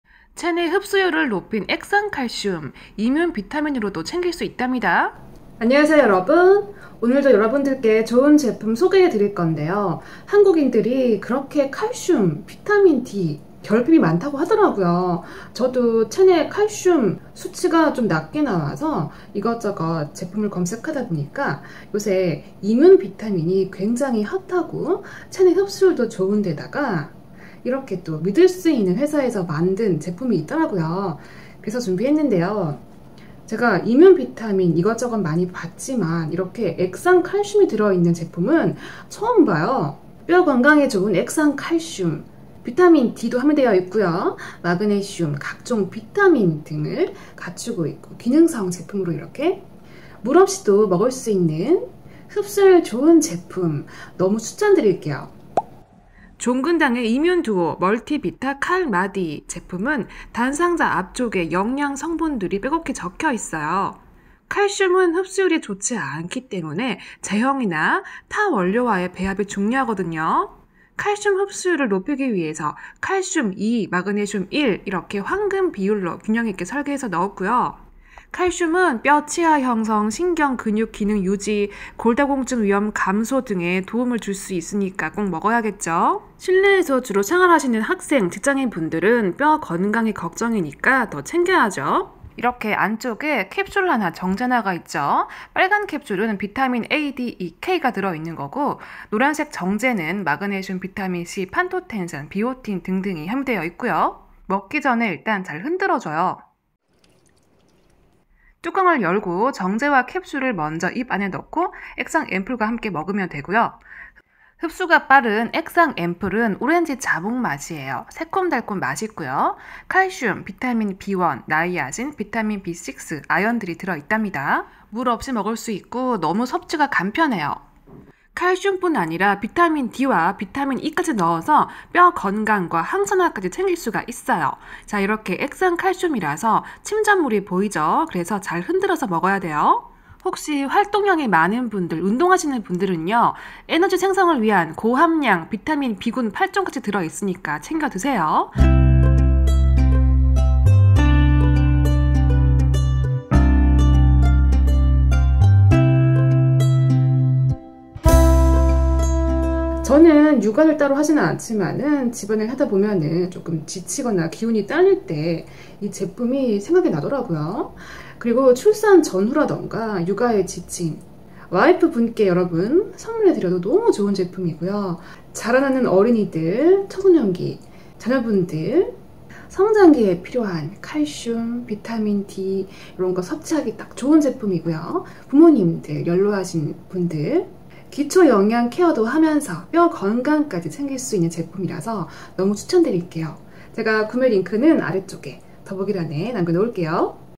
액상칼슘 비타민 추천 💊종근당 이뮨듀오 멀티비타 칼마디! 흡수율이 달라요
💊 종근당 액상칼슘 비타민 구매링크 남길게요